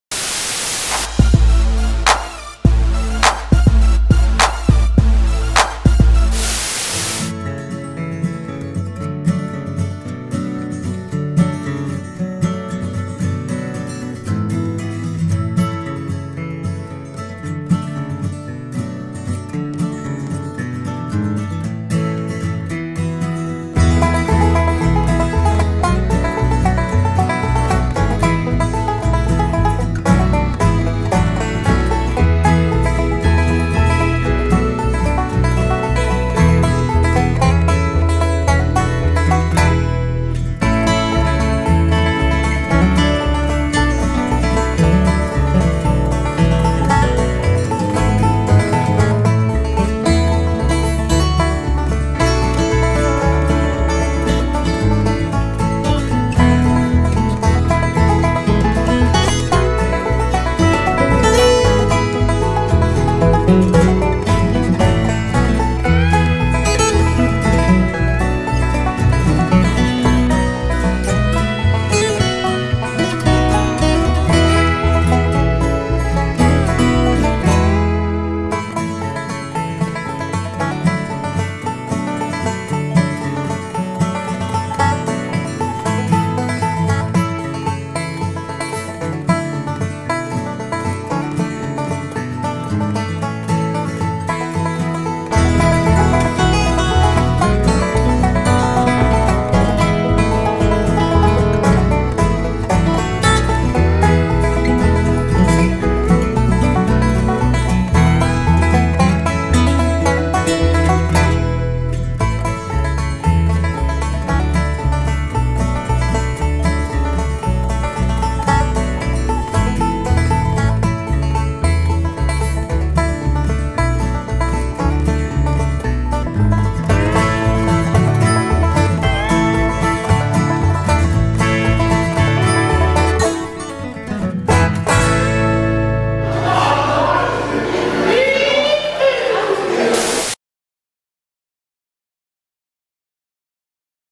Revivial Art Collective Opening
Siege and Jody from Revival Art Collective in Phoenix Arizona host a celebration to mark the opening of their new shop.  AT Imagery was there to capture some of those amazing and creative moments along with many smiling faces!